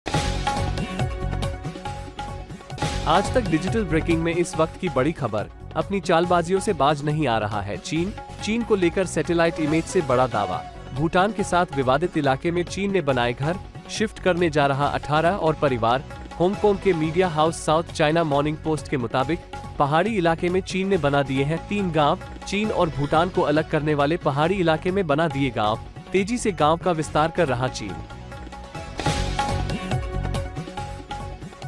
Breaking News: अपनी चालबाजियों से बाज नहीं आ रहा है China | China Built houses in Bhutan's Area
चीन ने एक बार फिर भूटान से लगने वाली सीमा पर अपनी बस्तियों का विस्तार करना शुरू कर दिया है. सैटेलाइट इमेज से यह बात सामने आई है कि चीन ने एक बार फिर विवादित जमीन पर बसे अपने गांवों की जमीन पर विस्तार किया है.